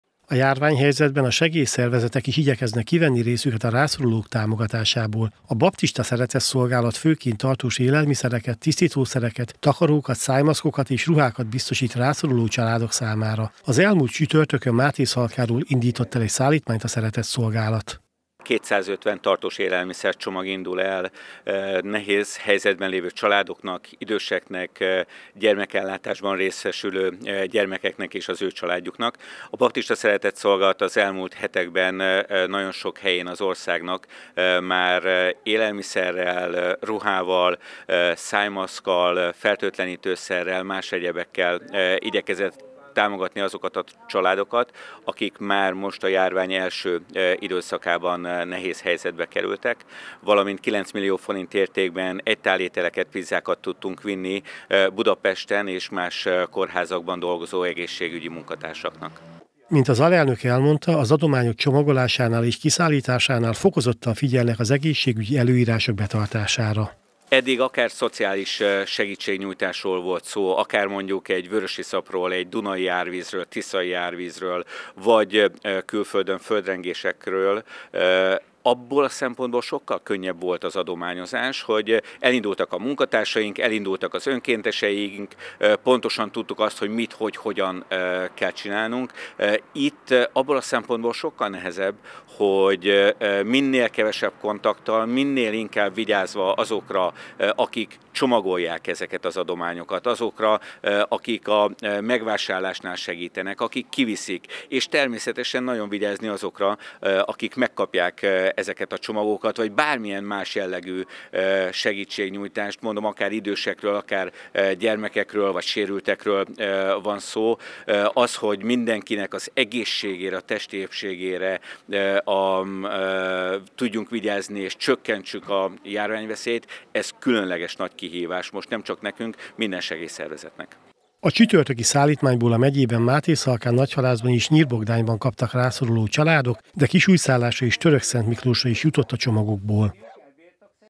Adomány a baptistáktól
Kétszázötven adag tartós élelmiszercsomagot indított útjára Mátészalkáról a Baptista Szeretetszolgálat.